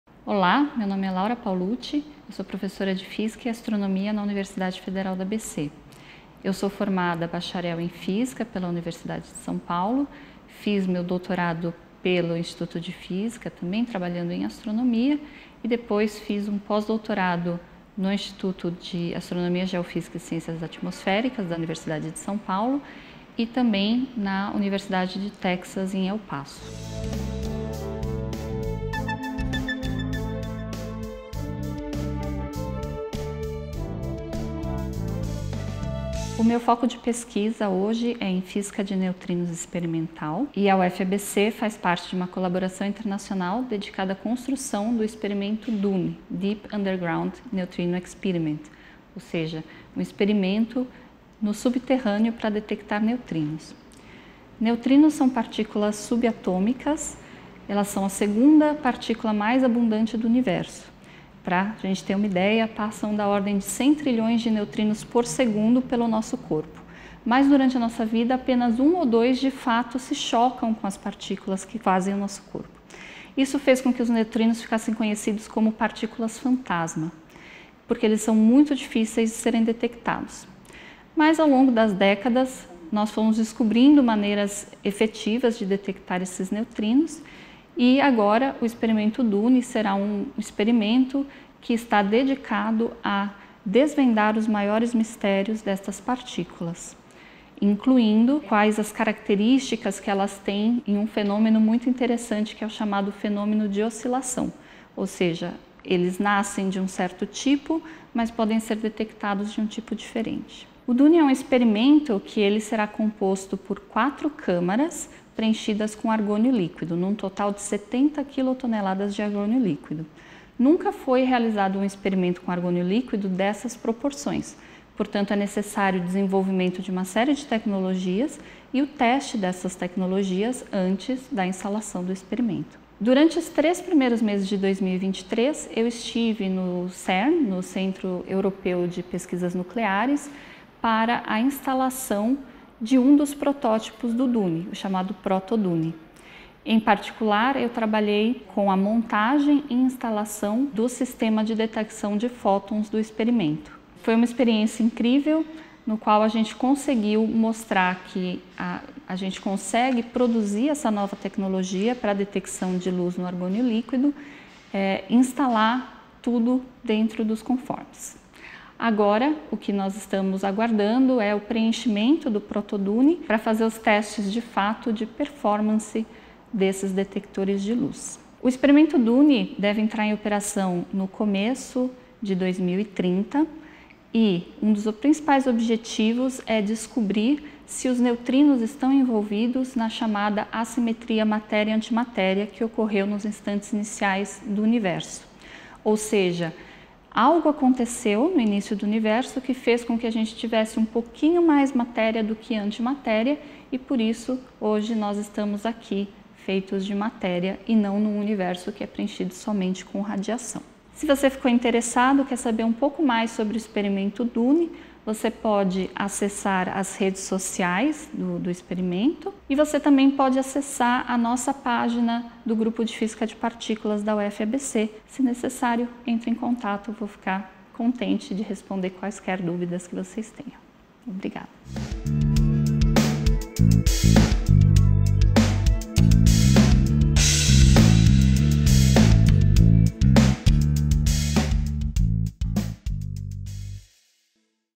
Pesquisas da UFABC colaboram em experimento internacional para detecção de neutrinos no subsolo
Laura Paulucci Marinho, docente no Centro de Ciências Naturais e Humanas da Universidade Federal do ABC (CCNH - UFABC), fala de pesquisas na área da física de neutrinos experimental, desenvolvidas em colaboração internacional no experimento Deep Underground Neutrino Experiment (DUNE). A iniciativa busca detectar essas partículas no subterrâneo e responder questões fundamentais sobre a natureza da matéria e a evolução do universo.

Projeto de extensão PJ053-2023 - PROEC/PAAE 2023